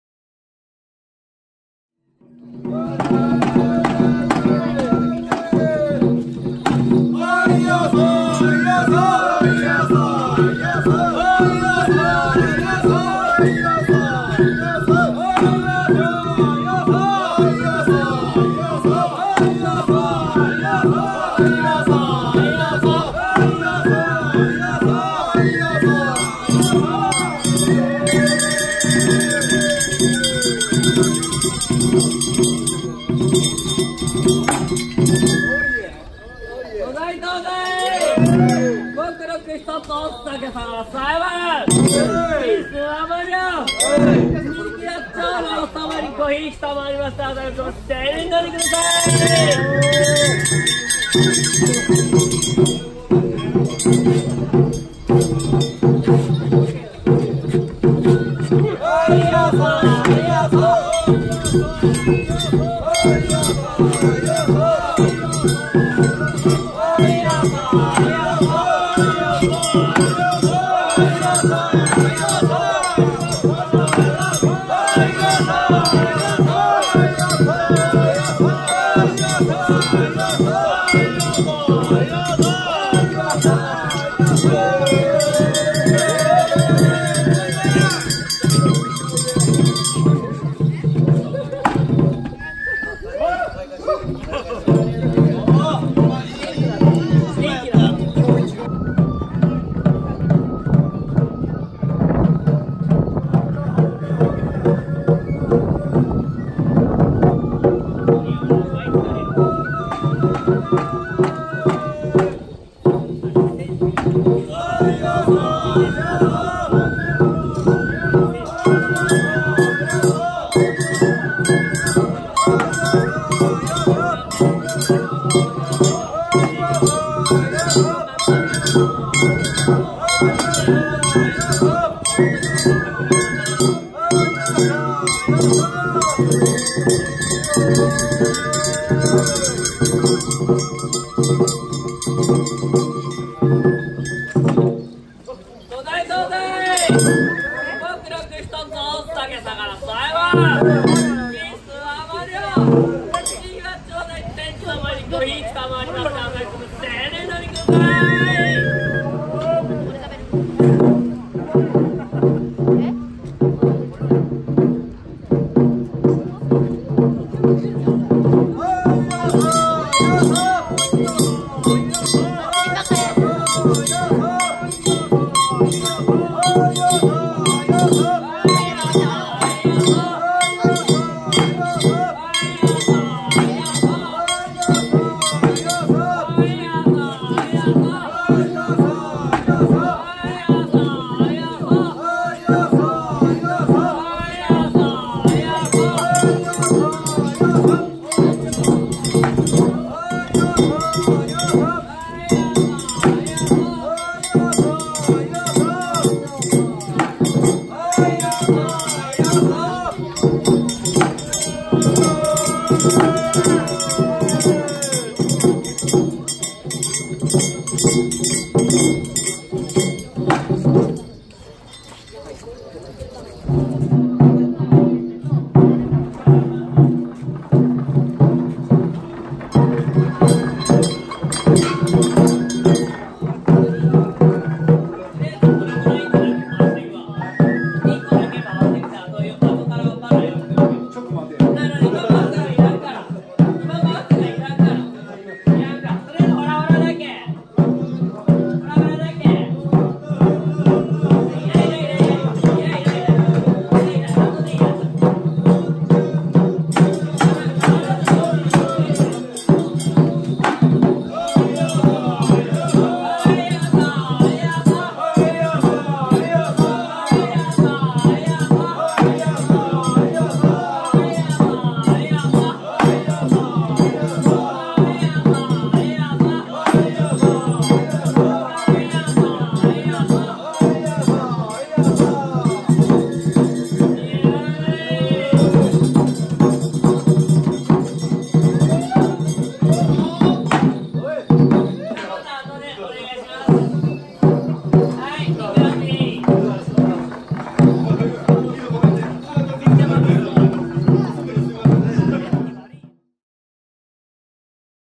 氷見 朝日北部の獅子舞 2019年 4K UHD/60p
昨年(2019年)春の氷見の獅子舞から朝日北部の獅子舞です。
2020年は春シーズンの氷見の獅子舞は、ほぼ行われない見込みで、昨年の春祭りの獅子舞から未公開のものを何本かＵＰさせていただきます(撮影位置や角度がよくなかったりして難ありのものもありますが、ご了承ください)。

撮影地: 富山県氷見市朝日北部
撮影日:2019年5月4日 午後2時過ぎ